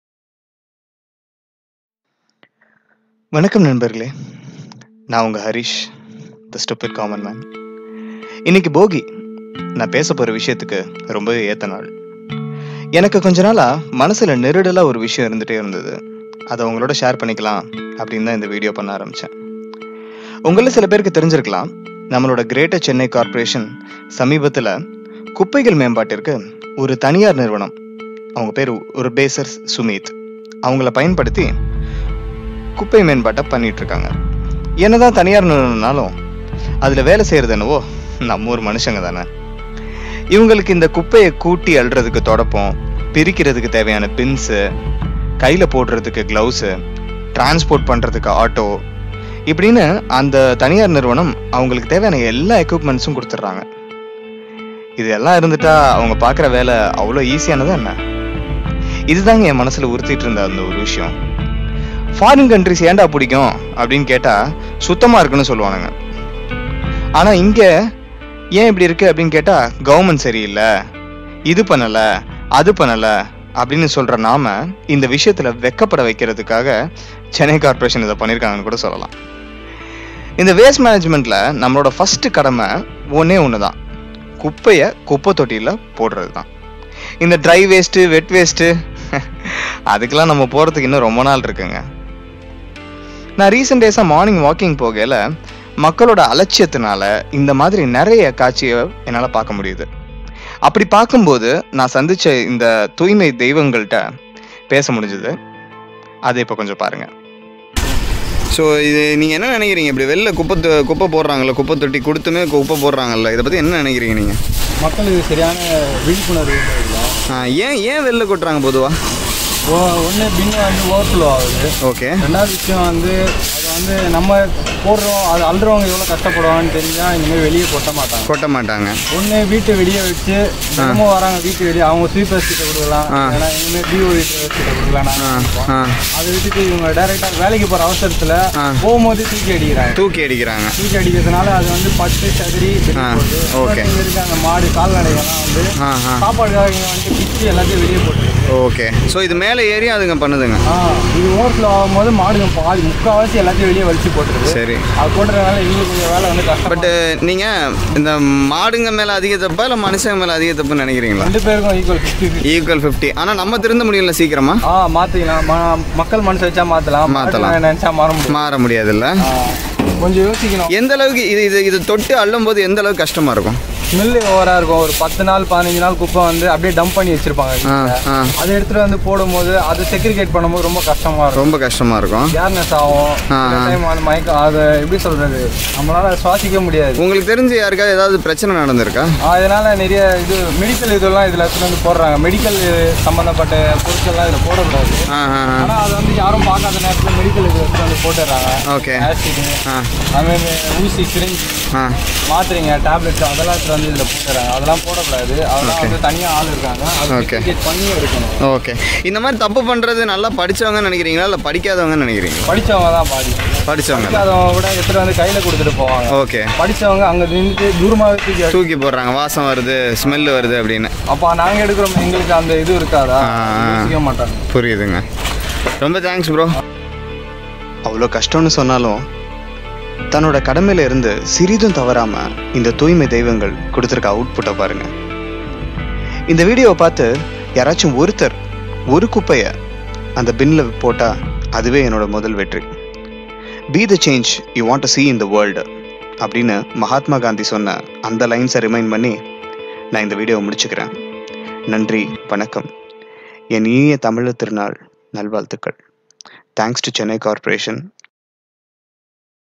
Kuppaiku Solvom Goodbye - Be a Responsible Citizen
My First Video on a Social Cause - Waste Management by Citizens